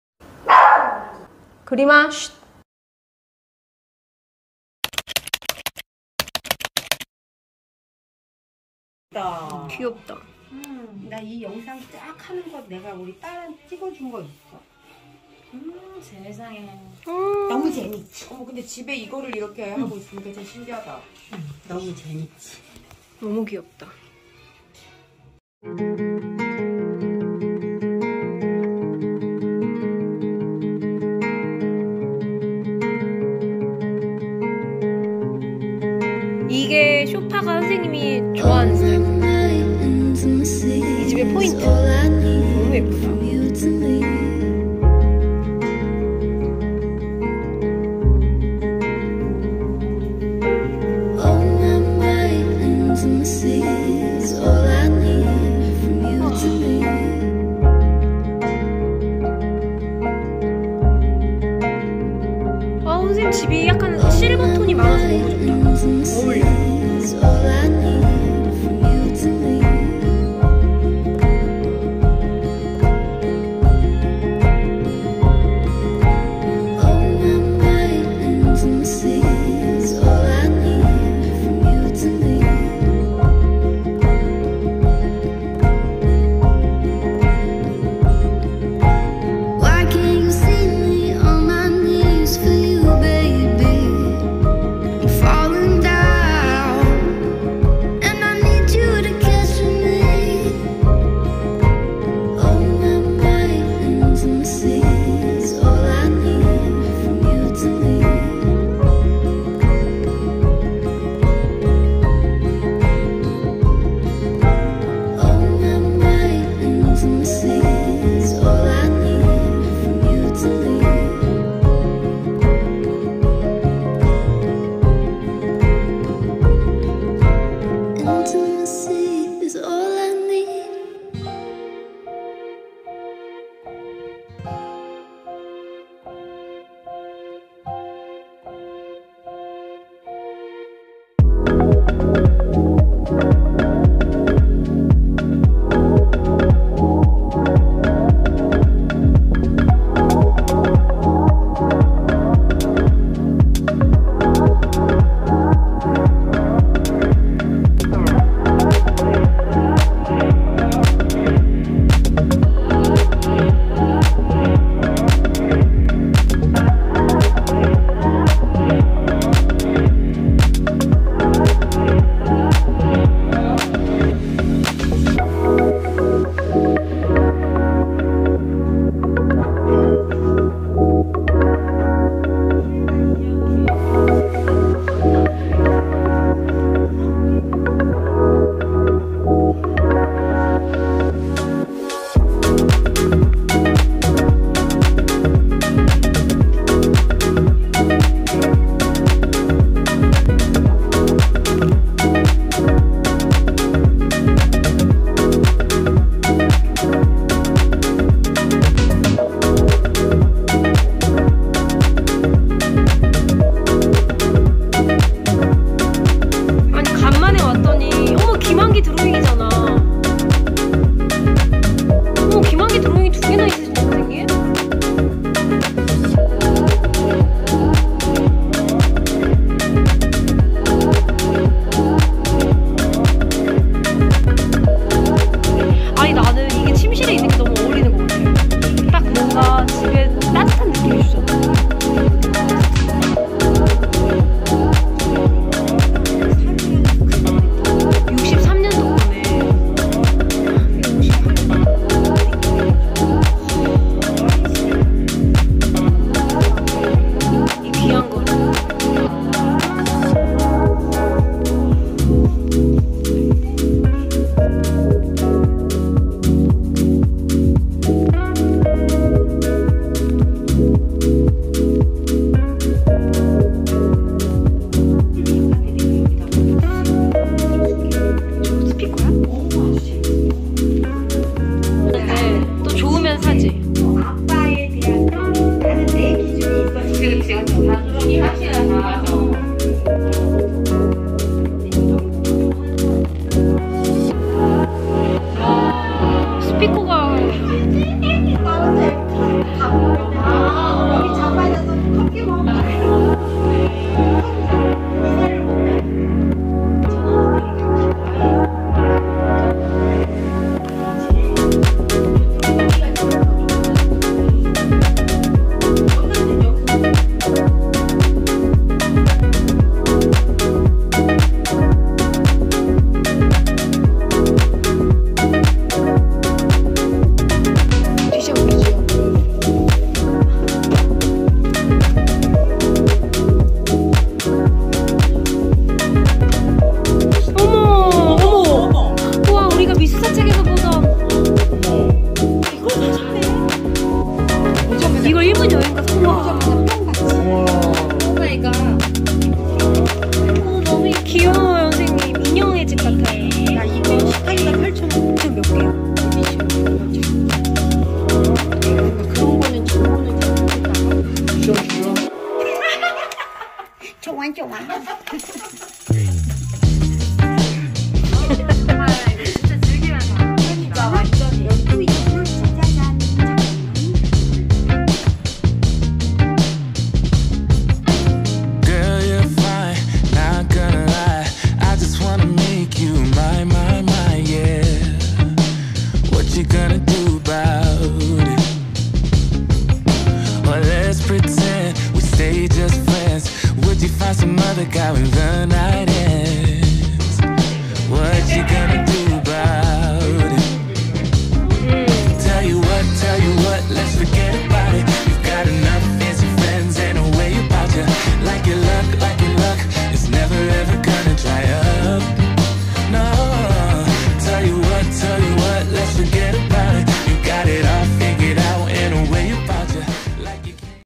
《아트컬렉터의 집》아파트에 사는 컬렉터는 어떻게 많은 작품과 함께 살아갈까?
ㆍ
ㆍ
ㅡ
인스타를 안하시지만 보기만 하시는
10년이상 컬렉팅을 한
컬렉터 의 집
ㅡ
내가 알고있는 작품이나 작가가 있는지 찾으셨다면
댓글로 소통해요!!!